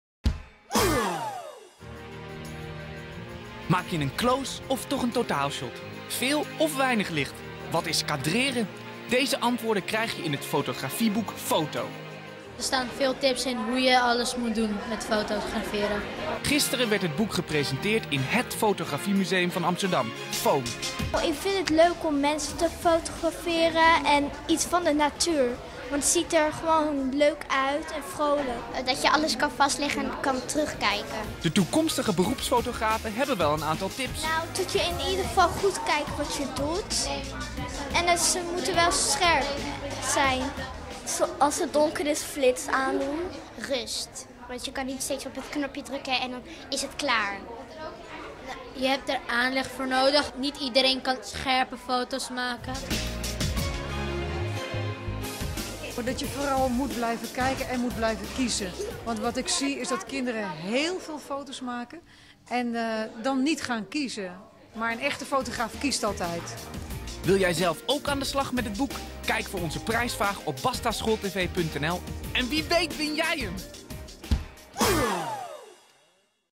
Bastanieuws: Fotografieboek Foto!
Compositie, contrast, camerastandpunt? Je moet er allemaal op letten als je gaat fotograferen. Voor tips, ideeën en uitleg is er nu fotografieboek Foto! Het boek werd gisteren gepresenteerd in fotografiemuseum Foam.